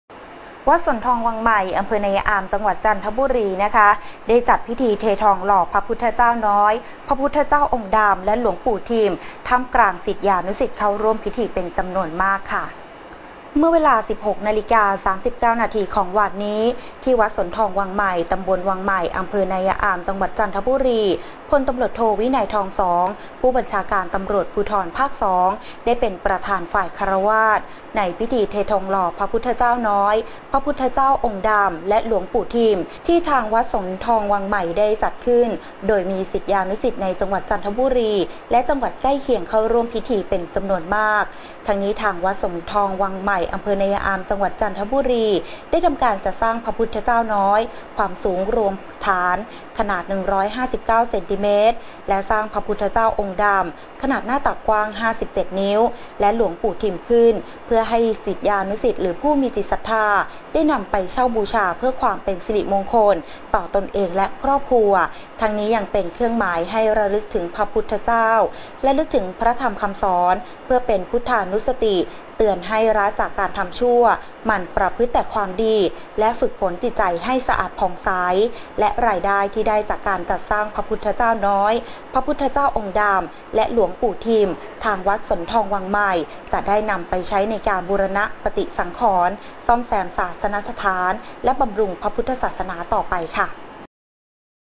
15-4-56 พิธีเททองหล่อ
วัดสนธิทองวังใหม่ อำเภอนายายอาม จังหวัดจันทบุรี จัดพิธีเททองหล่อพระพุทธเจ้าน้อย พระพุทธเจ้าองค์ดำ และหลวงปู่ทิม ท่ามกลางศิษยานุศิษย์เข้าร่วมพิธีเป็นจำนวนมาก